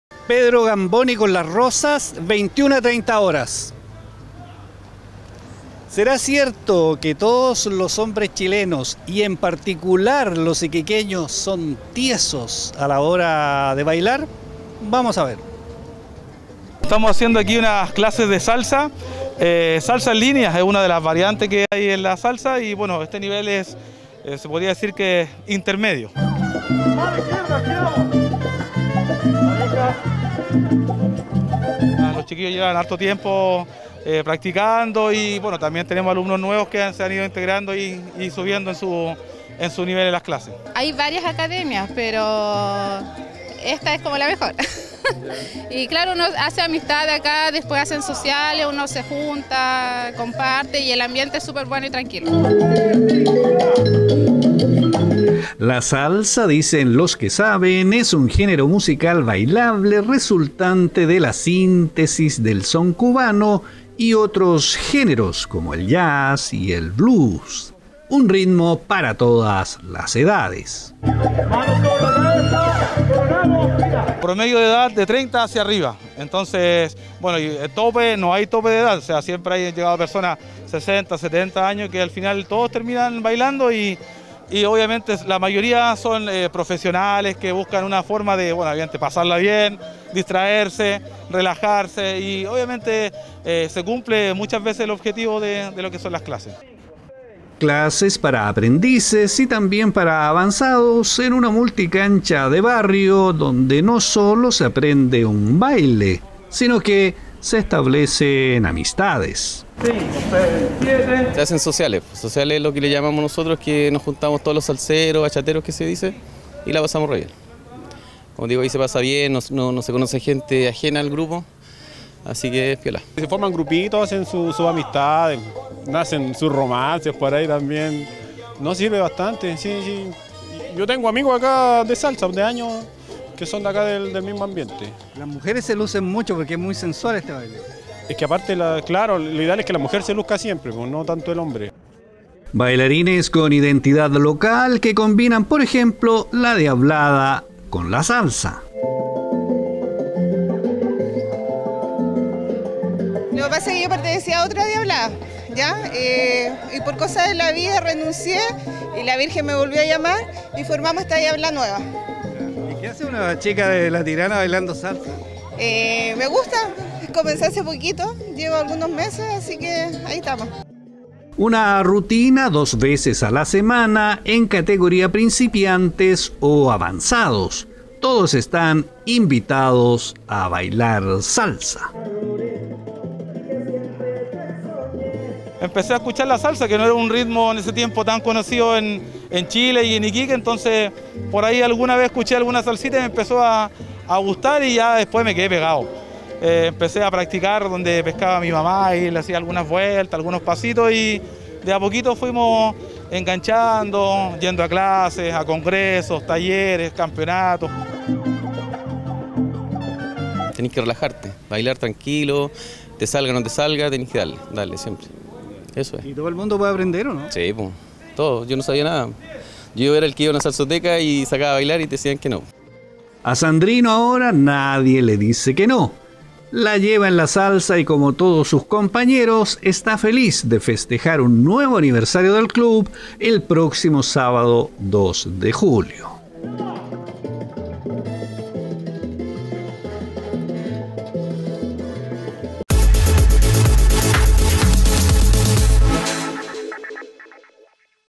Bailar salsa: un espacio para disfrutar la vida (24.06.2022) Iquique TV
La salsa, dicen los que saben, es un género musical bailable resultante de la síntesis del son cubano y otros géneros, como el jazz y el blues. Un ritmo para todas las edades. En Iquique, la academia Sabor Latino está próximo a cumplir 18 años. Hay que puro bailar. Y celebrar.